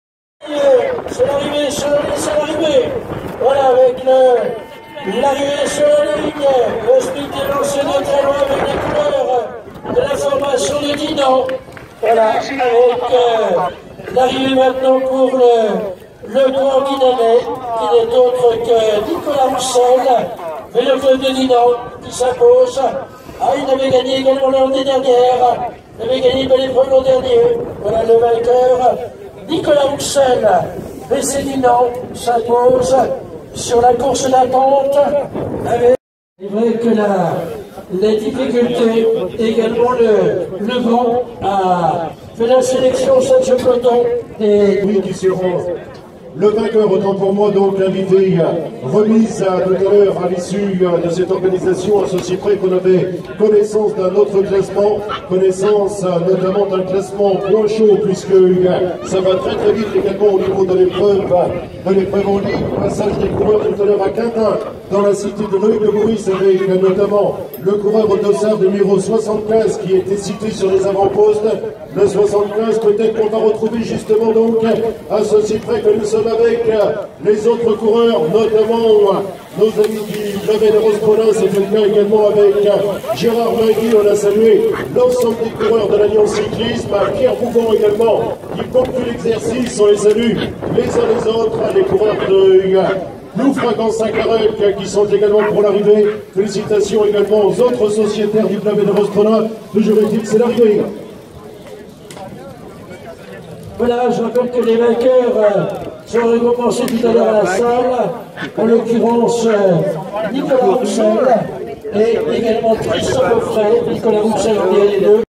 Plaintel : Arrivée Course Pass'cyclisme D1/D2 26 février 2017
1er Nicolas Rouxel ( VC Dinanais)
2ème Julien LE GUENNEC Julien ( TEAM PODIOCOM JANZE=